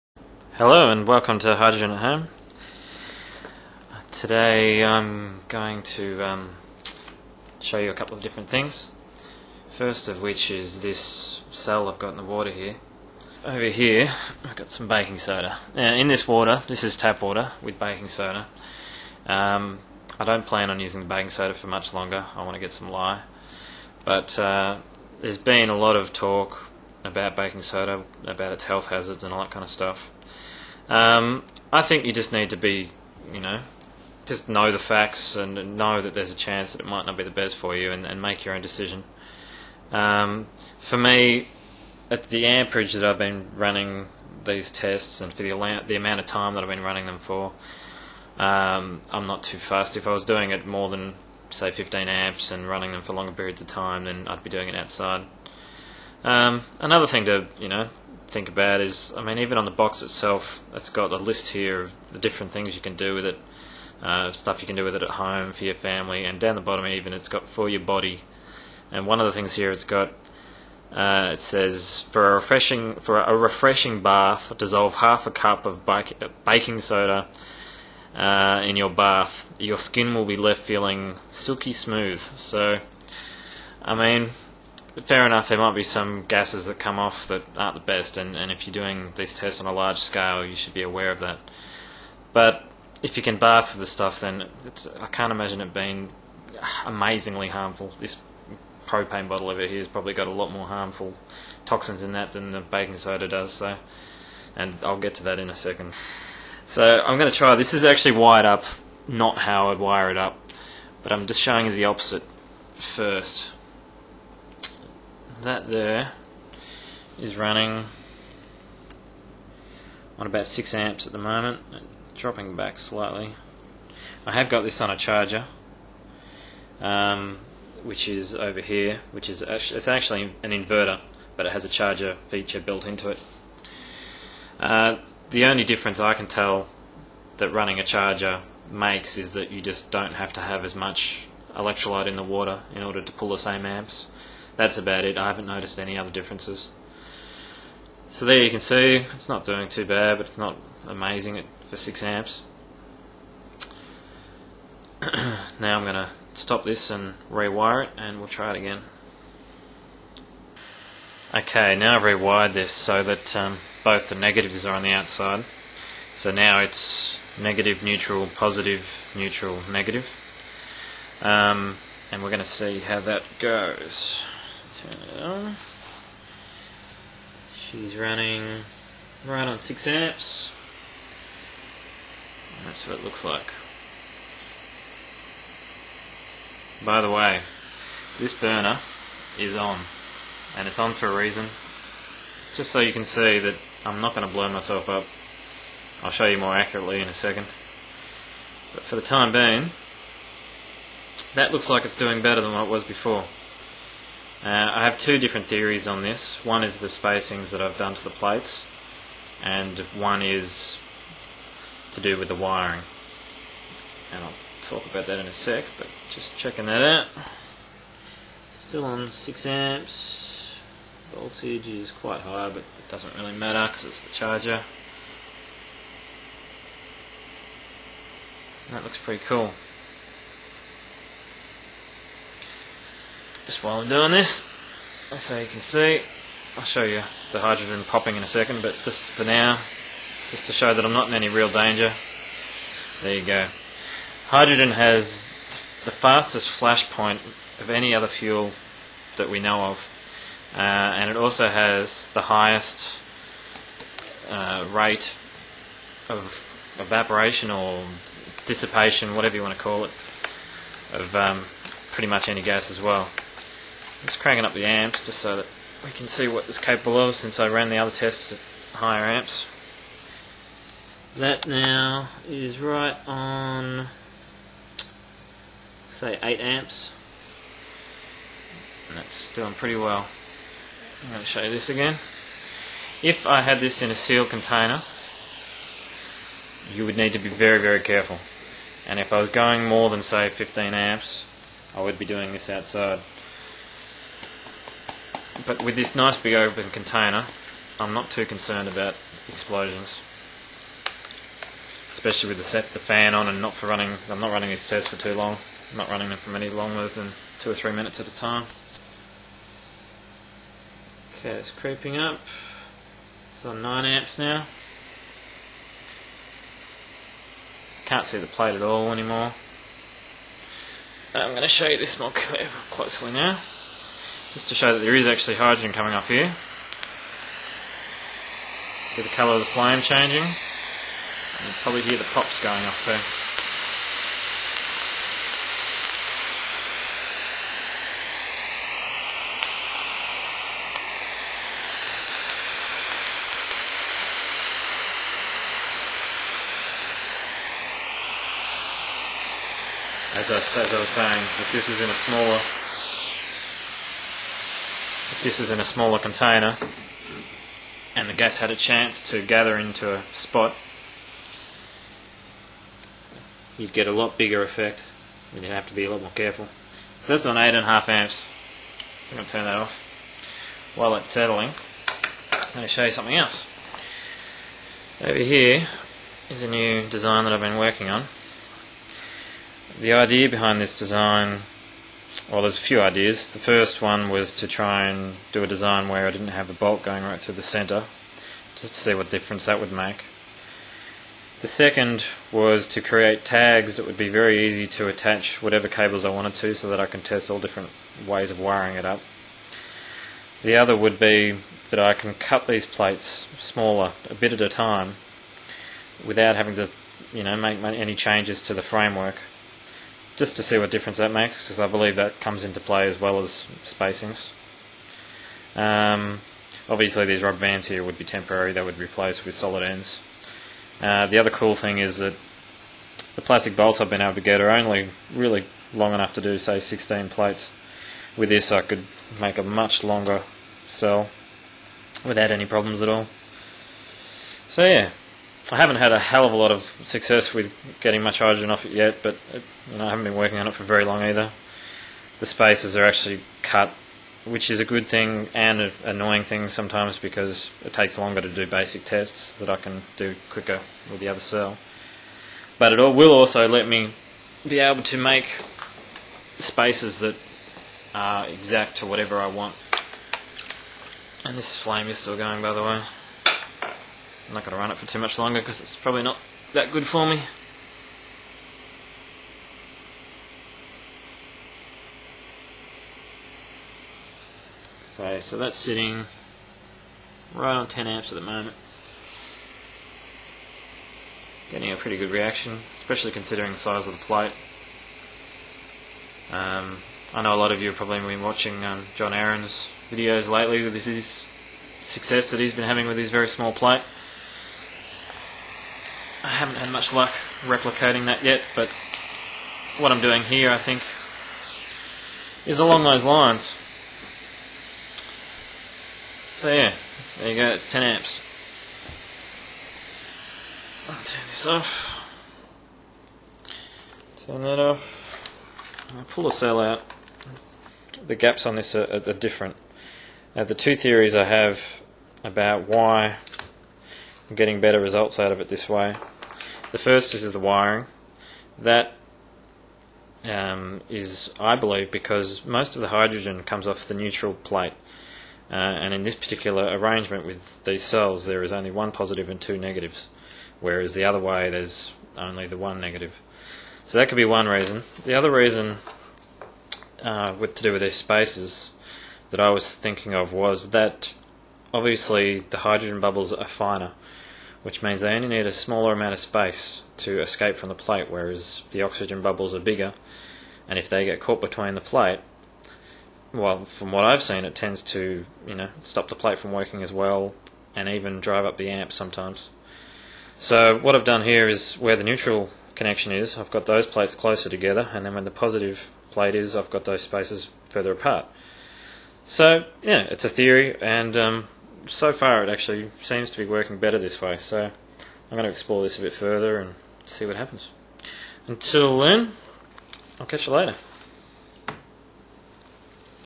Hydrogen at home (plate gaps and new cell design)#10
This video shows a test using uneven spacings between the plates and talks about baking soda, explosions, and shows my new cell design in detail.